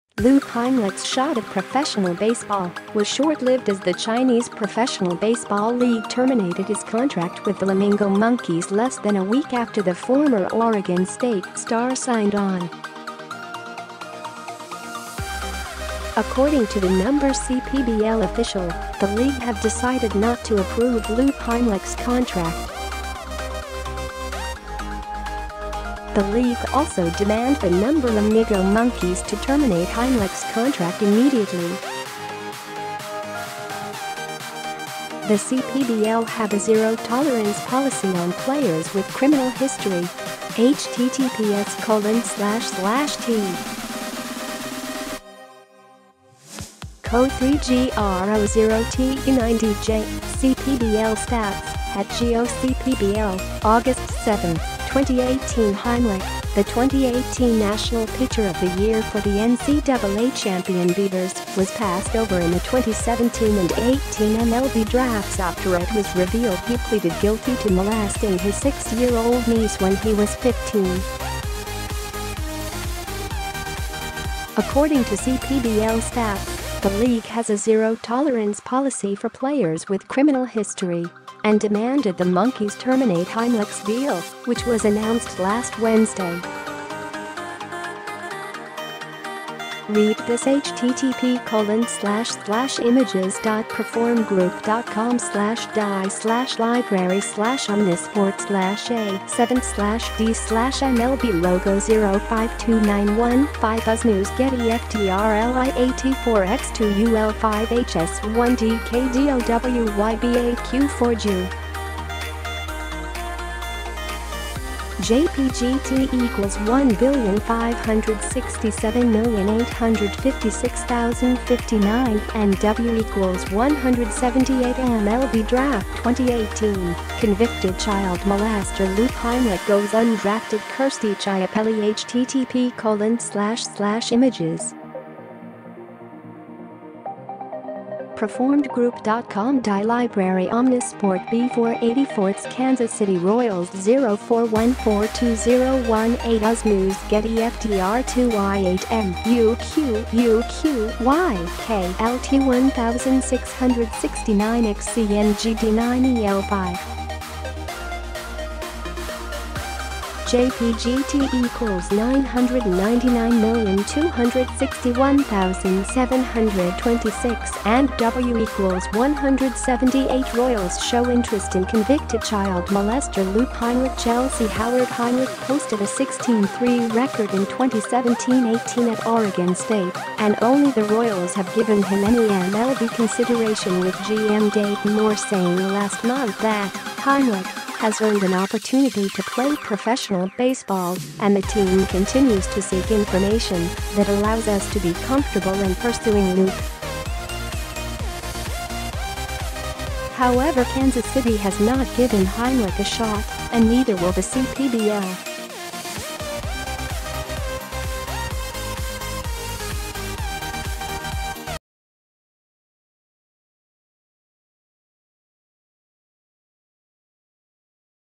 Former Oregon State star Luke Heimlich's contract terminated by Chinese baseball league
Luke Heimlich’s shot at professional baseball was short-lived as the Chinese Professional Baseball League terminated his contract with the Lamingo Monkeys less than a week after the former Oregon State star signed on. According to the CPBL official, the league have decided not to approve Luke Heimlich's contract.The league also demand the Lamigo Monkeys to terminate Heimlich's contract immediately.The CPBL have a zero tolerance policy on players with criminal history. CPBL STATS (@GOCPBL) August 7, 2018  Heimlich, the 2018 National Pitcher of the Year for the NCAA champion Beavers, was passed over in the 2017 and '18 MLB Drafts after it was revealed he pleaded guilty to molesting his 6-year-old niece when he was 15.  According to CPBL Stats, the league has a zero-tolerance policy for players with criminal history and demanded the Monkeys terminate Heimlich’s deal, which was announced last Wednesday. Read This MLB Draft 2018: Convicted child molester Luke Heimlich goes undrafted Kirstie Chiappelli Royals show interest in convicted child molester Luke Heimlich Chelsea Howard  Heimlich posted a 16-3 record in 2017-18 at Oregon State, and only the Royals have given him any MLB consideration with GM Dayton Moore saying last month that “(Heimlich) has earned an opportunity to play professional baseball” and the team continues to “seek information that allows us to be comfortable in pursuing Luke.”  However, Kansas City has not given Heimlich a shot, and neither will the CPBL.